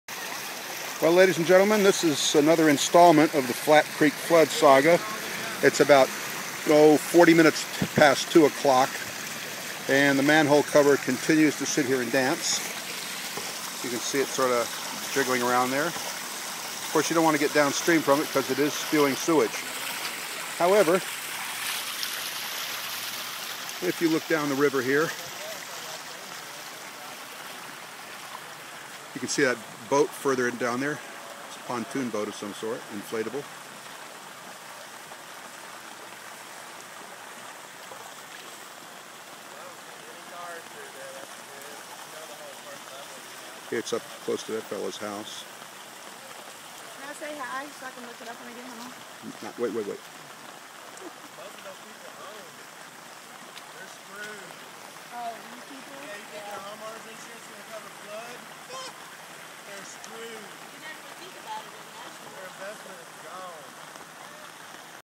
Flat Creek Flood 4
Flooding of Flat Creek in the Bellevue area of Nashville along Todd Pries Drive on Saturday, May 2, 2010 around 2:40 PM.